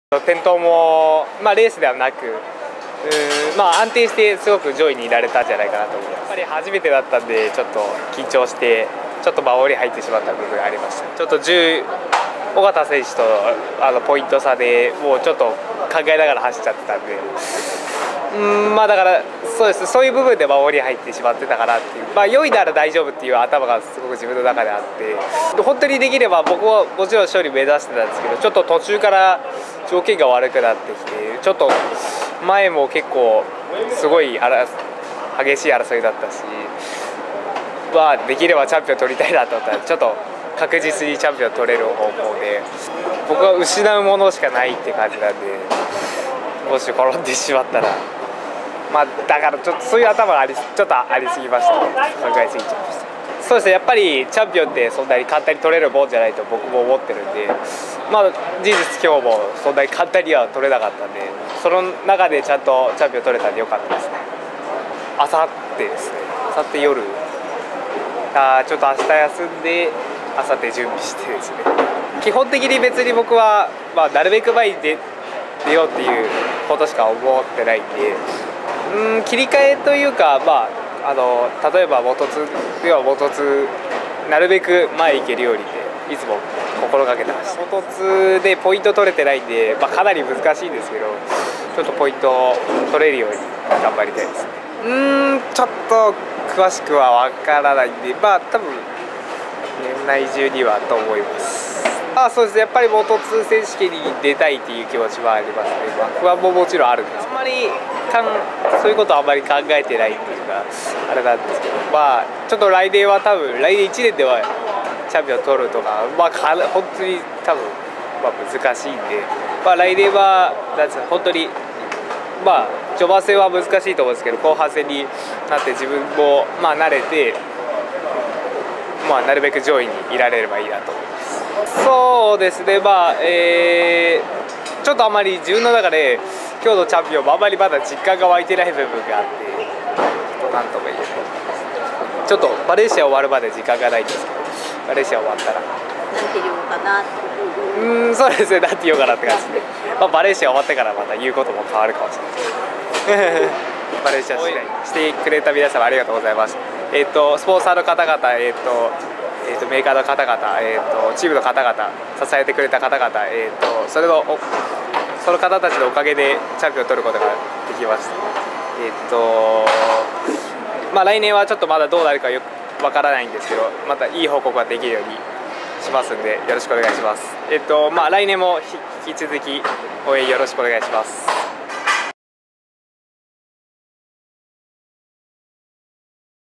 野左根が全日本JGP2クラスチャンピオンを獲得
来季、ロードレース世界選手権モト2クラスへ参戦予定の野左根航汰が１１月３日、鈴鹿サーキットで行なわれた最終戦で２０１３年全日本ロードレース選手権JGP2クラスのチャンピオンを獲得した。激しいバトルが繰り返された中、冷静なレース運びで４位に入っての獲得だった。